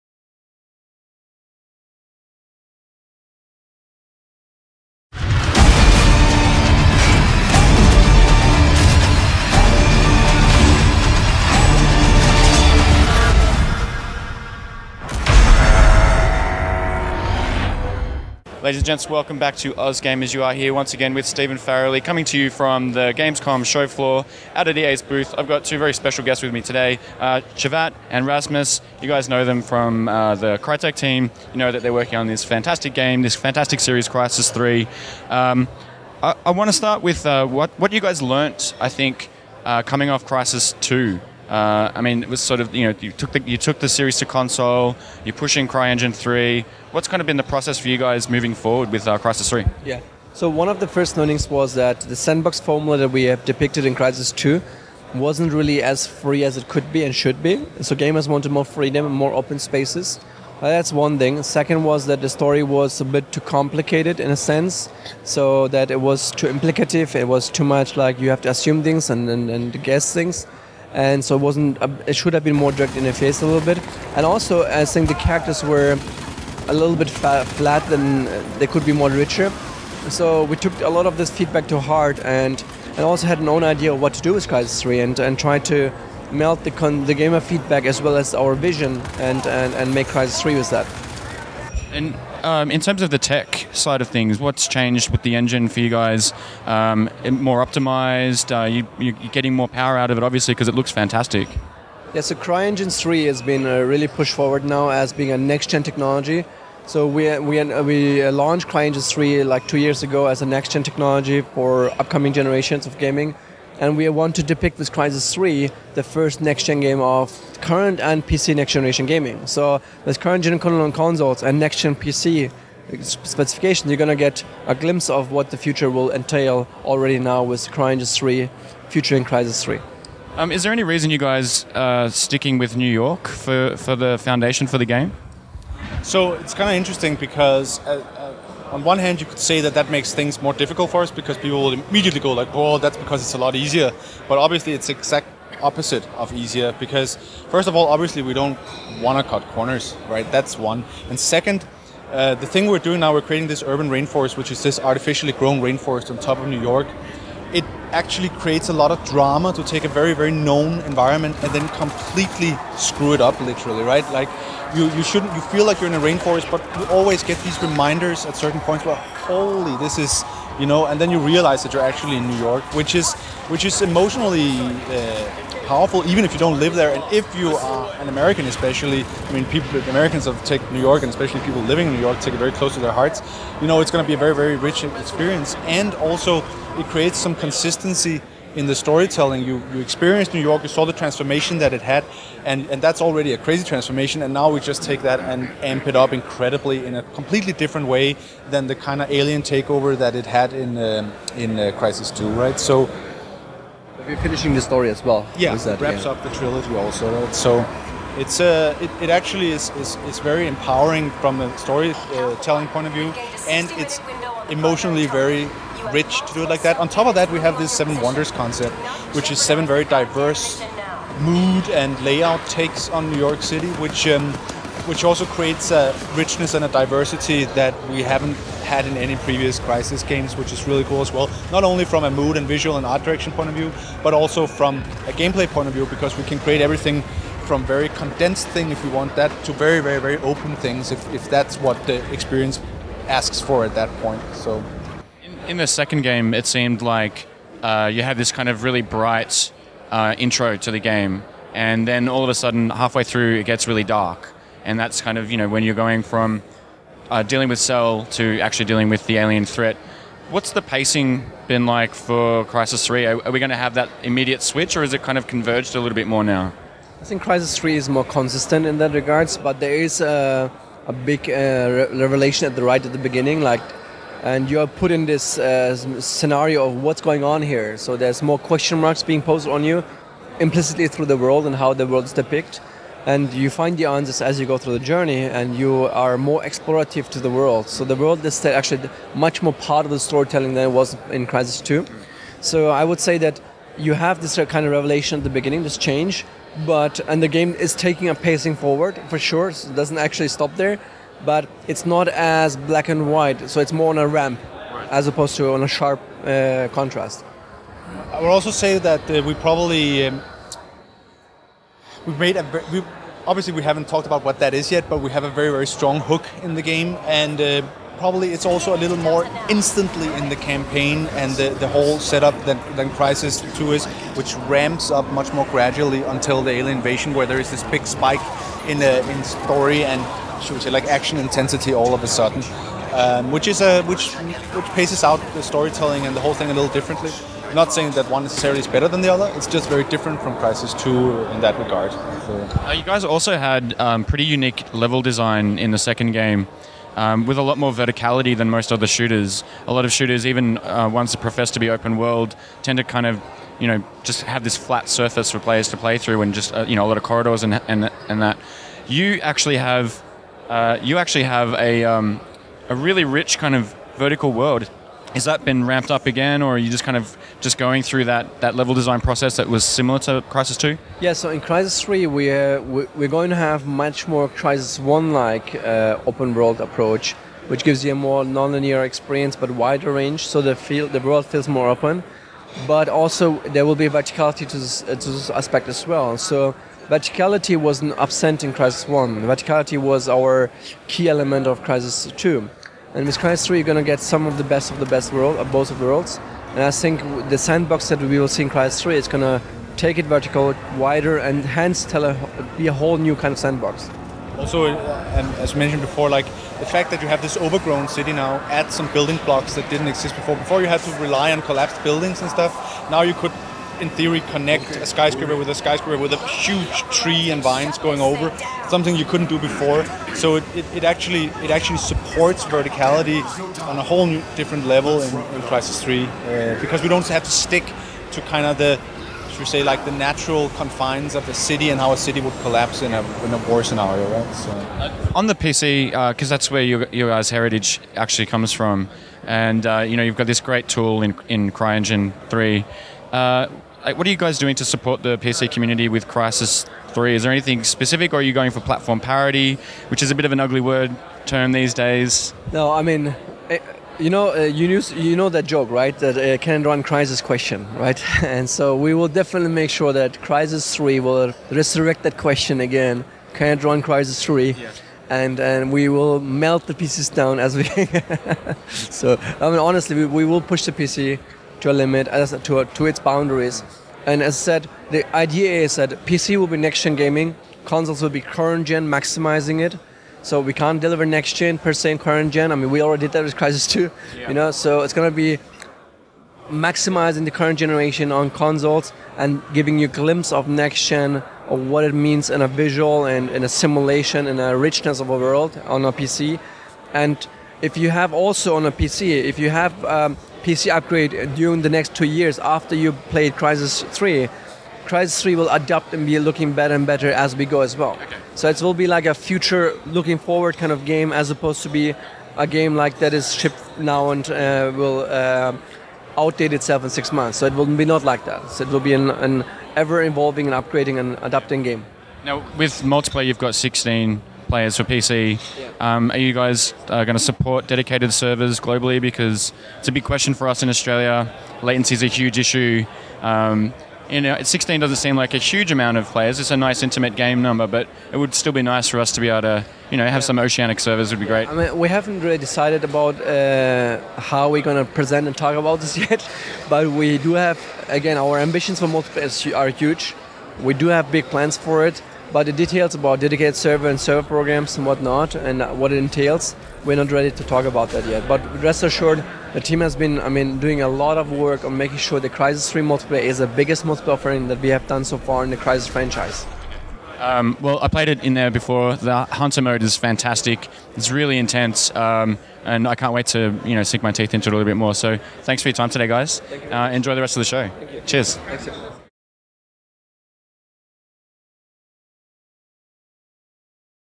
AusGamers GamesCom 2012 Crysis 3 Developer Interview with Cevat Yerli
At the 2012 GamesCom expo in Cologne, Germany, AusGamers catches up with Crytek CEO Cevat Yerli and Director of Creative Development Rasmus Hoejengarrd for a chat about the incredible looking, upcoming first person shooter Crysis 3.